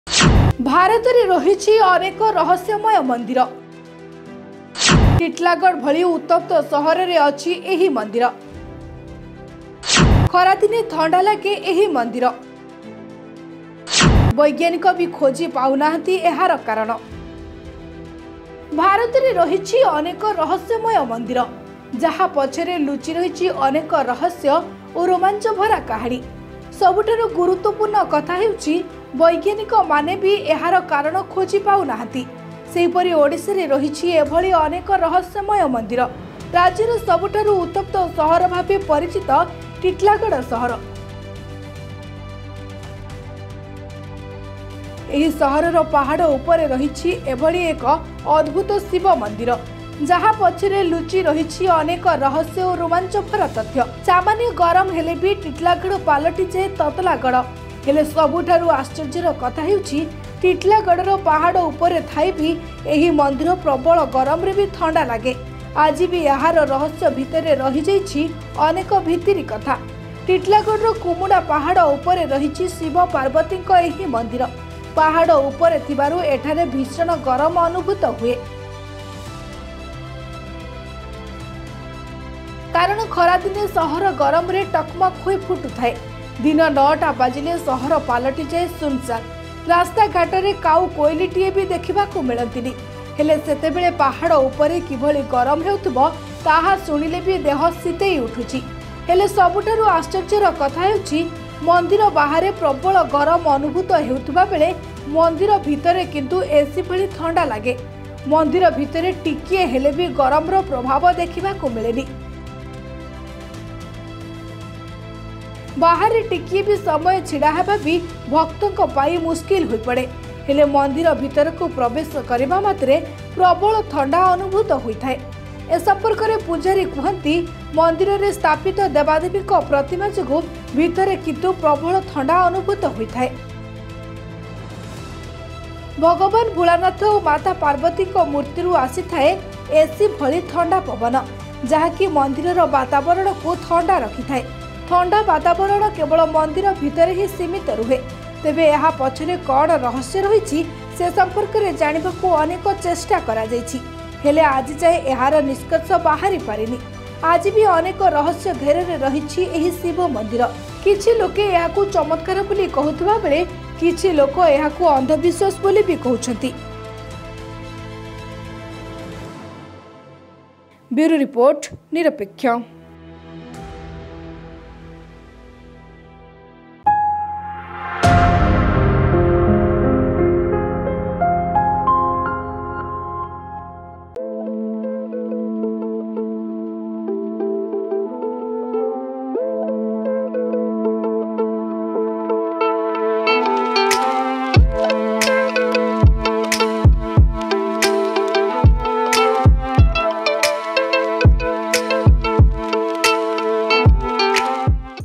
ଓଡ଼ିଶାର ରହସ୍ୟମୟ ଶିବ ମନ୍ଦିର, ଖରାଦିନେ ବି ଥଣ୍ଡା ଲାଗେ.Mysterytemple Temple Nirapekshya News
NIRAPEKSHYA NEWS | ODISHA NEWS | MIRACLE PLACE
ବୈଜ୍ଞାନିକ ବି ଖୋଜି ପାଉନାହାଁନ୍ତି ଏହାର କାରଣ..
🔆MORE UDATE🔆